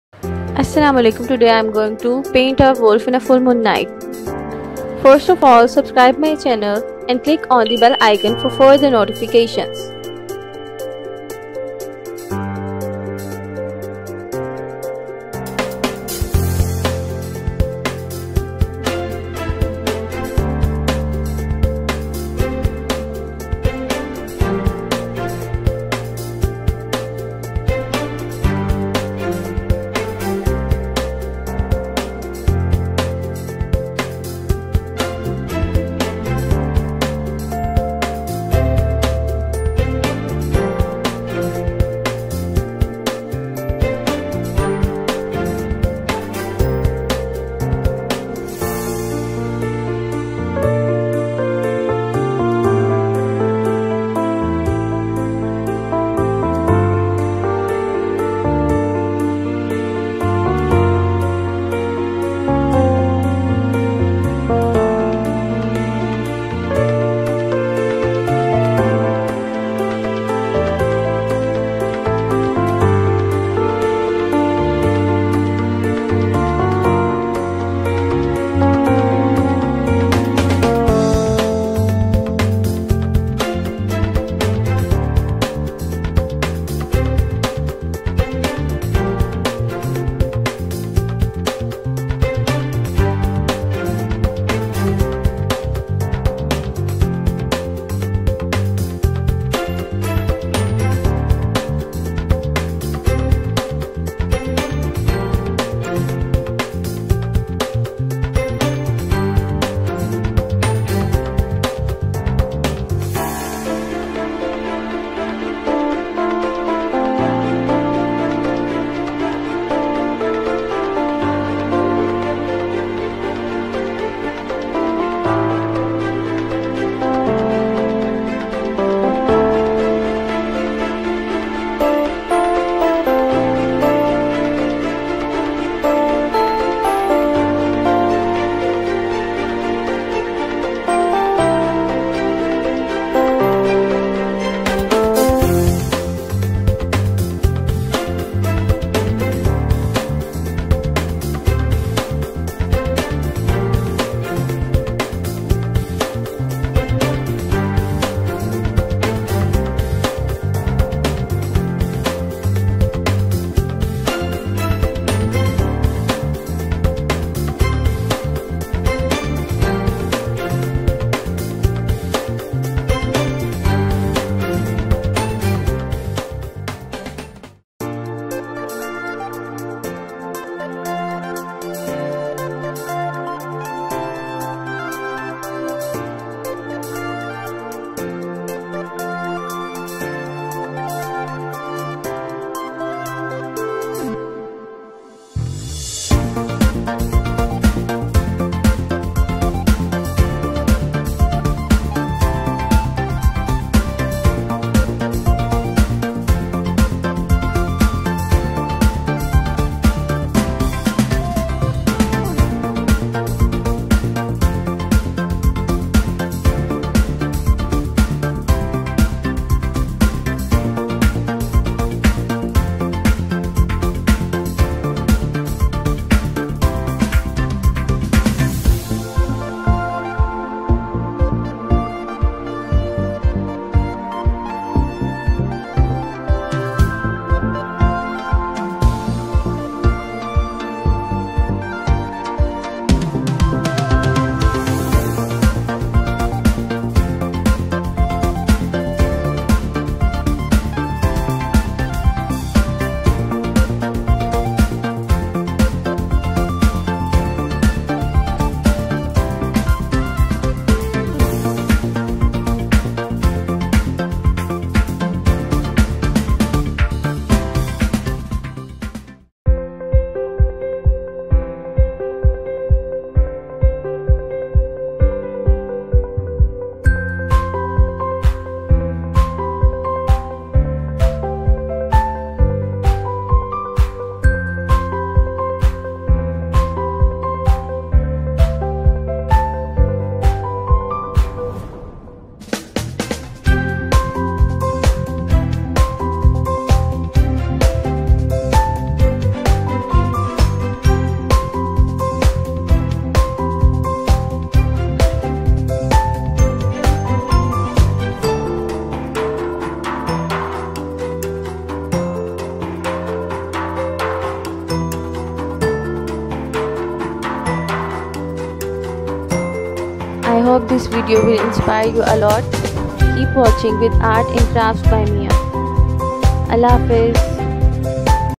Painting of wolf in a Full Moon night Tutorial by MIA| Full moon effects🌕🐺|ART&CRAFTS BY MIA
I decided to paint a Wolf in a Full Moon night as there are some reasons 🌙

Wolf are friendly animals
Seems dangerous but are adorable
They care
They spread love
They train their kids like humans do
Really faithful

Despite these, in this painting full moon with red shade has emerged and wolf is howling in front of the moon.

Trees at night are giving out a terrific look

See the effects

KEEP SHARING FRIENDS